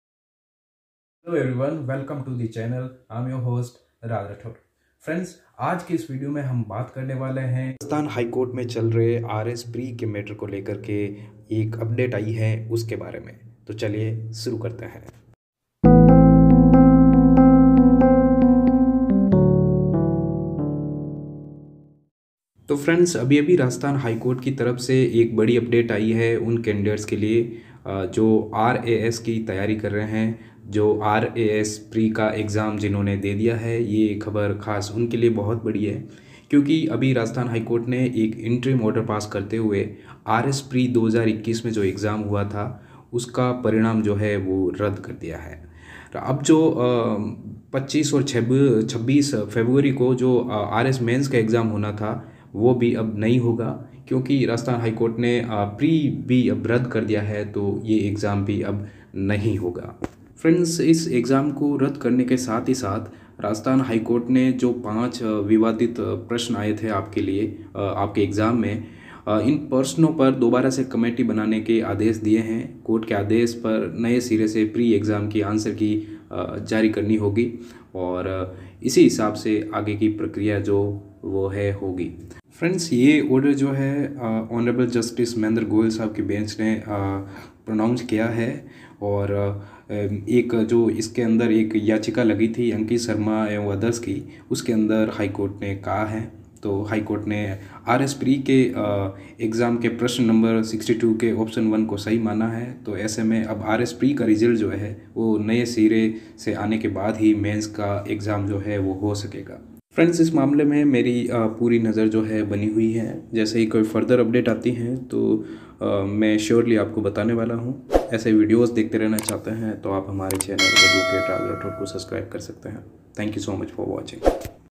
RAS pre 2021 exam रद्द राजस्थान हाई कोर्ट का ऑर्डर।
ras preexam 2021 exam रद्द राजस्थान हाई कोर्ट का ऑर्डर।

Contact as for legal help
Our callme4app id
Adv.RaajRathore@cm4

Thank you so much so much!.